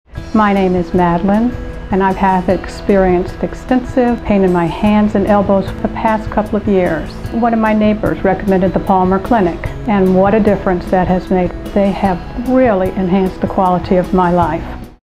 Madelyn Patient Profile :15 (PO)
Madelyn tried every medical specialist she could to heal the pain in her arms.  She didn’t find any relief until she visited Palmer Clinic, where she received chiropractic care and cold laser therapy, which has made all the difference for her. to make your appointment today.